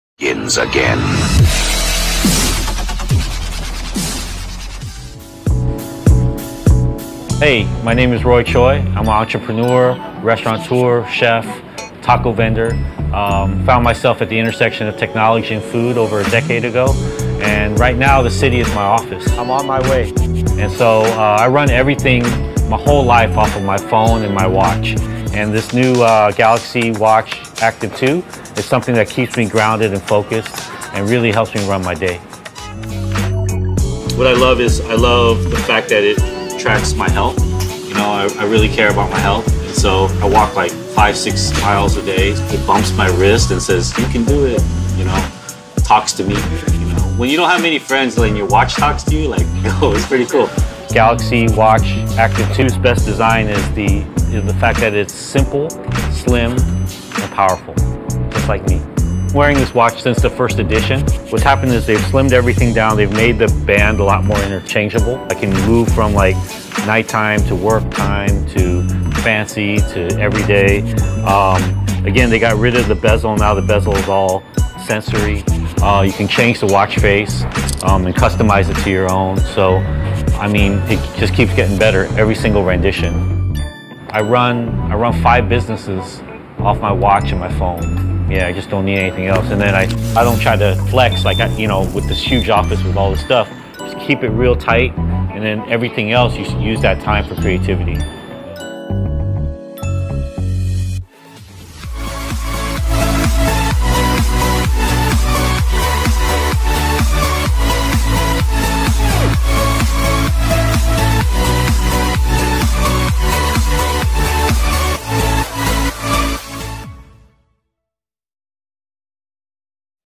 Galaxy Watch Active 2 : Official Introduction with (Roy Choi)
Introducing the new GalaxyWatchActive2. Designed to give you smart advice to better manage of all areas in life, including your wellness. Roy Choi talks about how the watch Active2 helps him balance work and life.

This Vedio is from officially by Samsung.